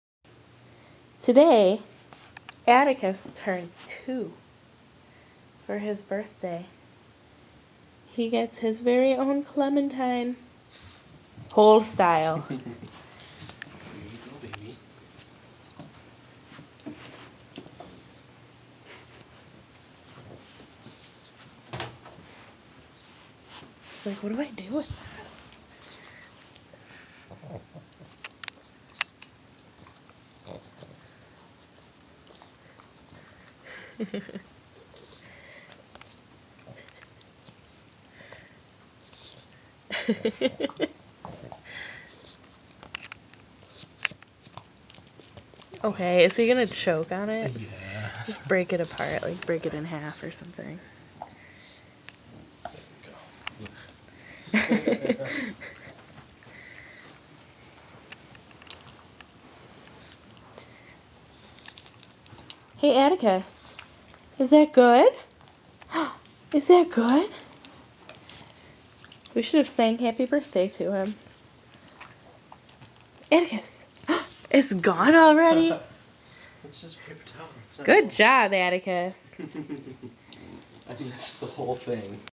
AtticusBirthday2
Eating a whole clementine for his birthday treat